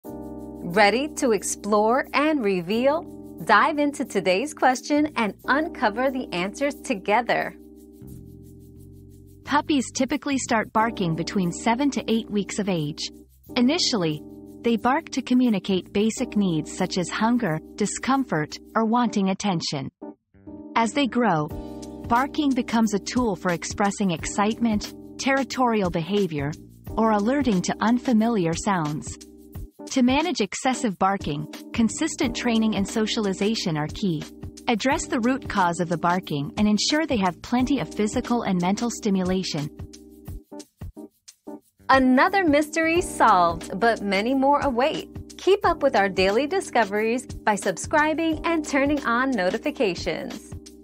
When do puppies start barking and why?
Bark Talk: When Do Puppies Start Barking? 👉 Puppy Barking 101 👉 Learn all about when puppies start barking and why in this informative video. Discover the reasons behind puppy barking and how to manage it effectively through training and socialization.

Laura S. Harris (2024, May 22.) When do puppies start barking and why?
    🌐 AskAbout.video